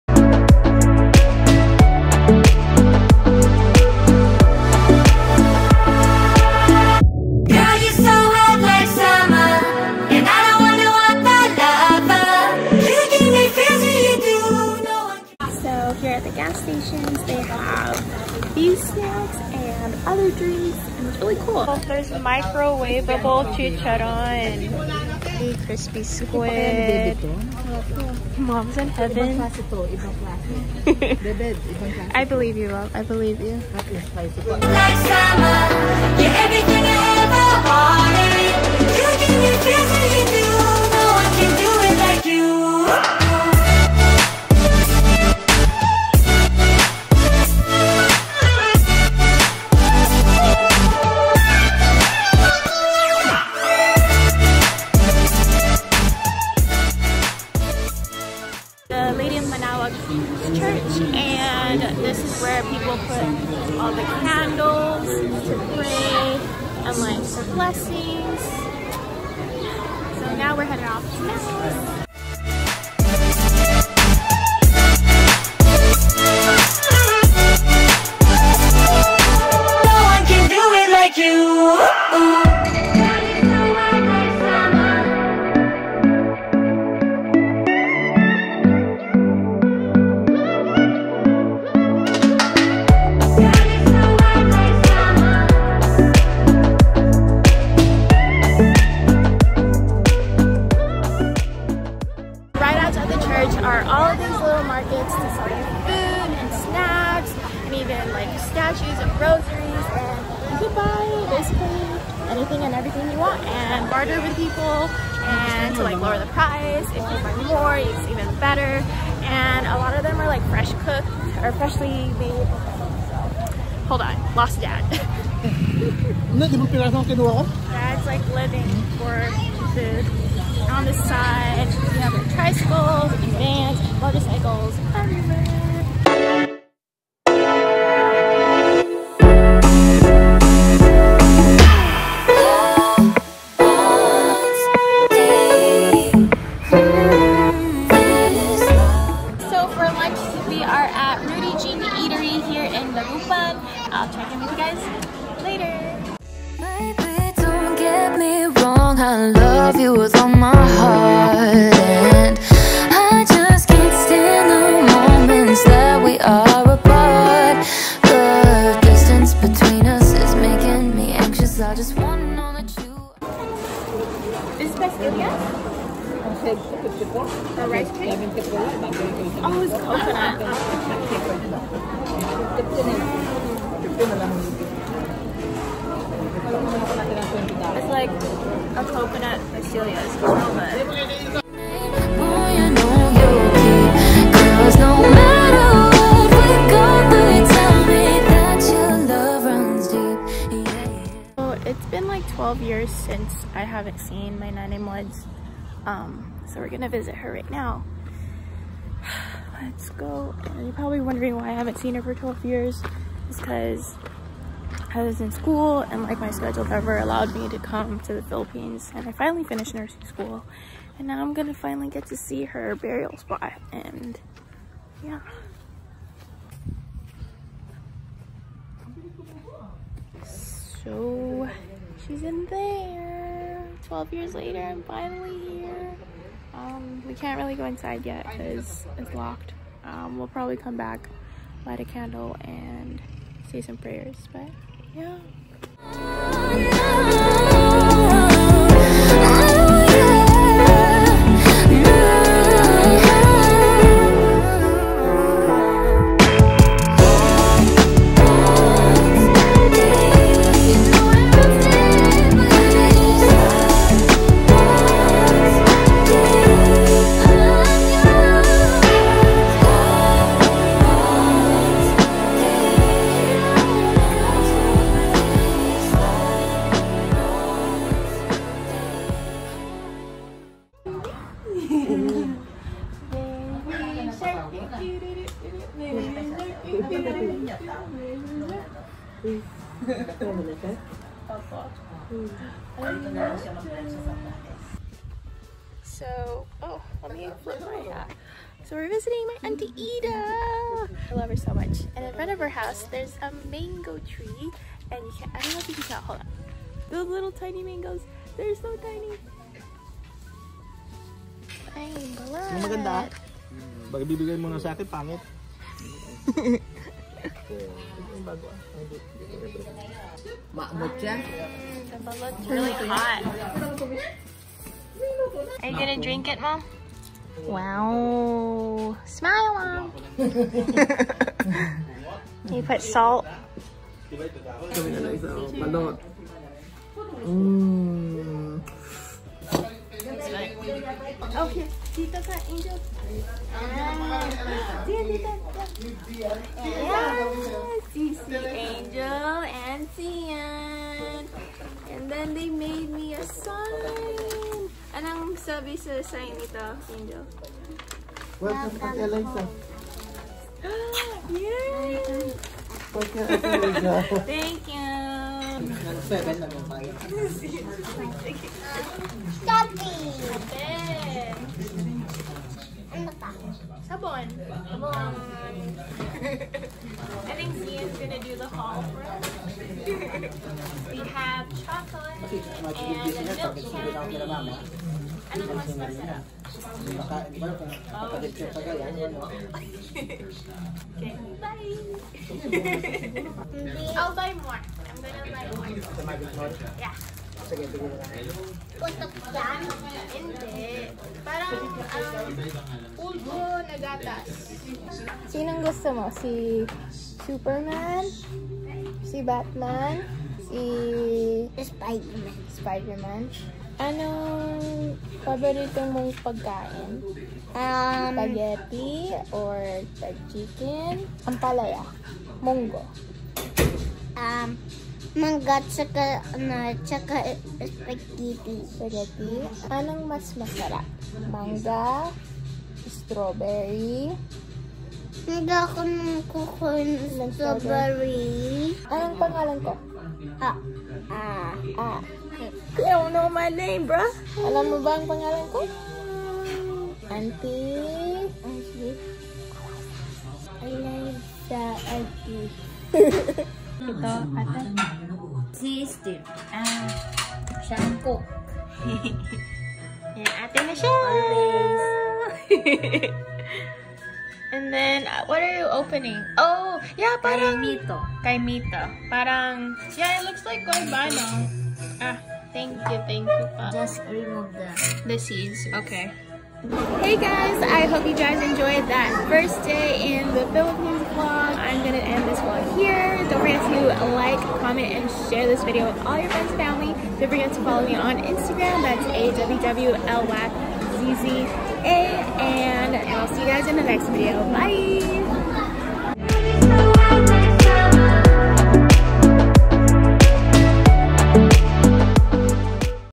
VLOG 2 - Where to go in the Philippines 2020 (It's been so long since I've seen my family!)
Hi everyone! For time clarification:
This trip was in end of January/early February 2020 before quarantine was put in place end of March for California. We had no idea this would be our last family trip of 2020, thankfully we came back home healthy. I have time to edit 20+ vlogs and I am not traveling out of the country at the moment.